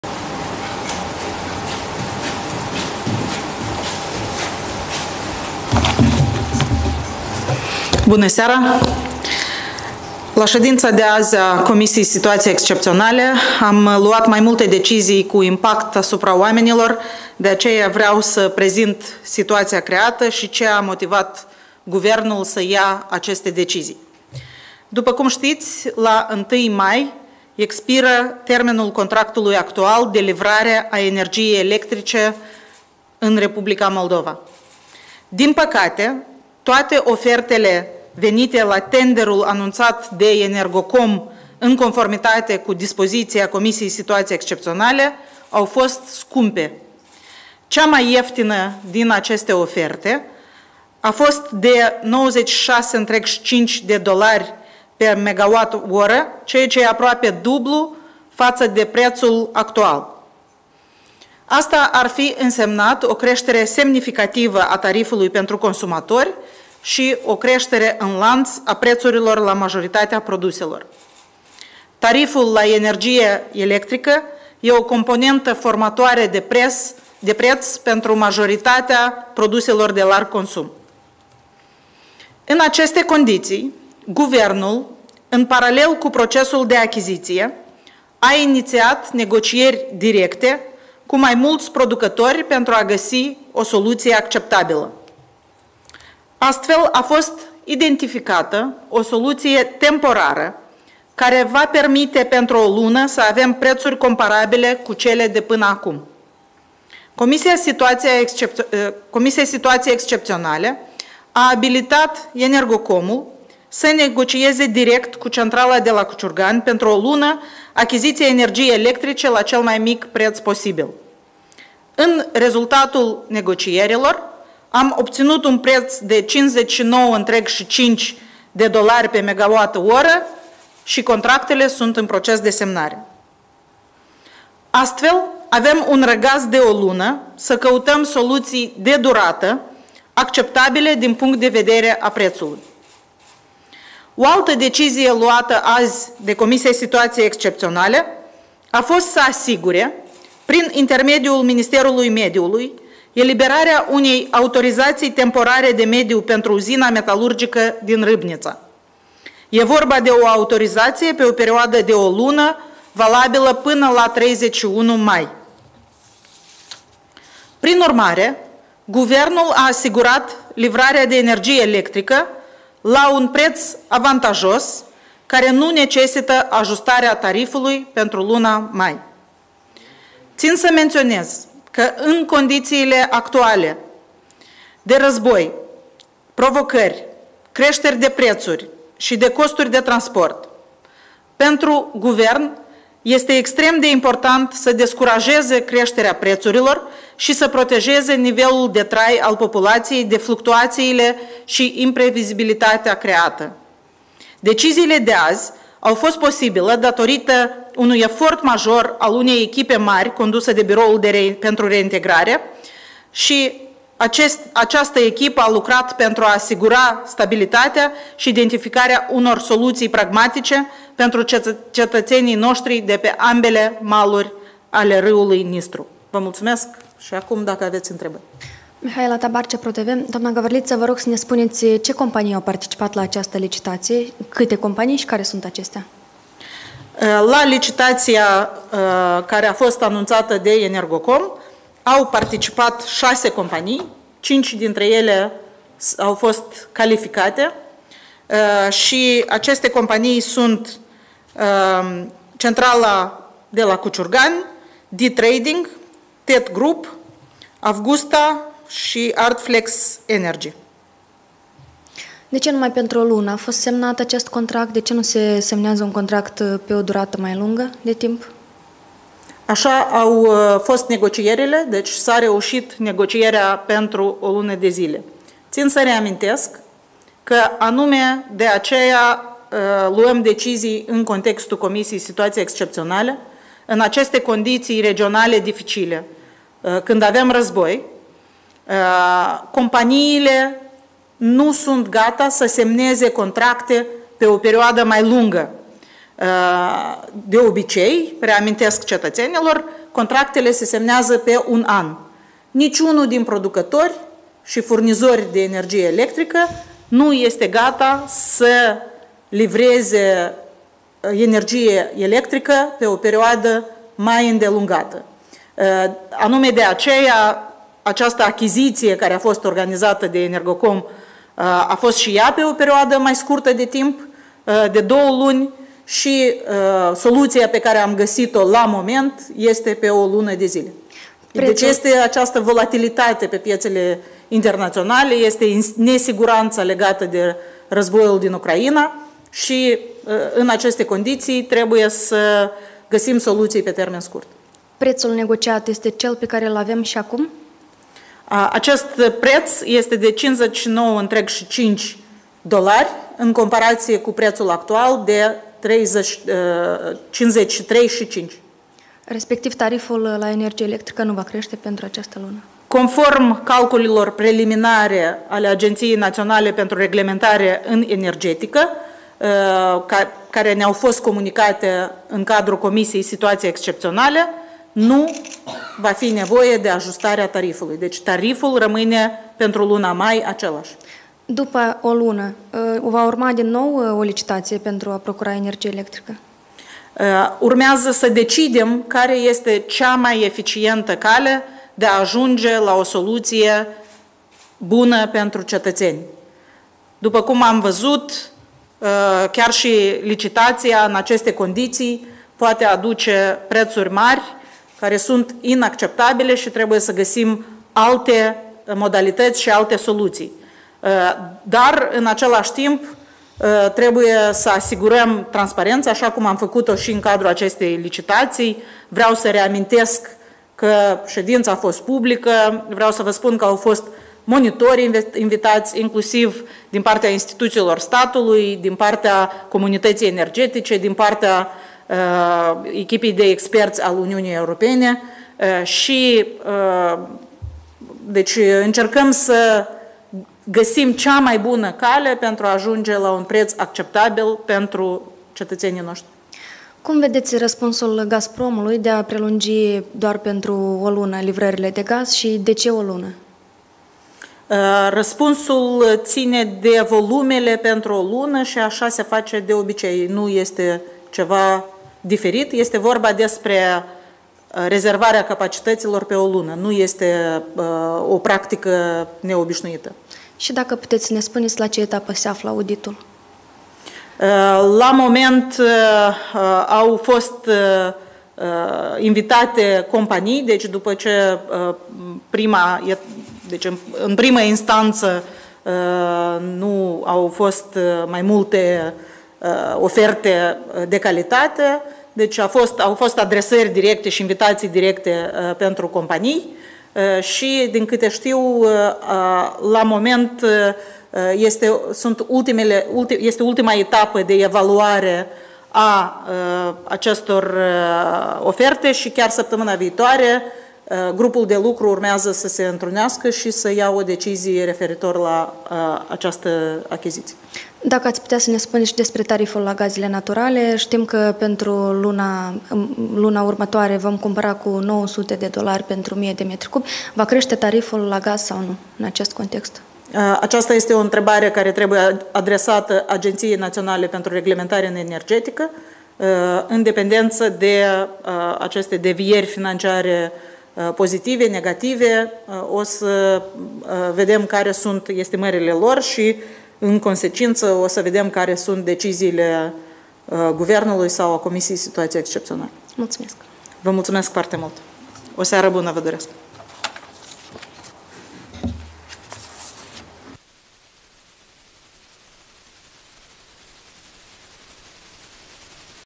Conferință susținută de prim-ministra Republicii Moldova, Natalia Gavrilița, după ședința CSE
Conferință de presă susținută de prim-ministra Republicii Moldova, Natalia Gavrilița, după ședința Comisiei pentru Situații Excepționale - 29 aprilie 2022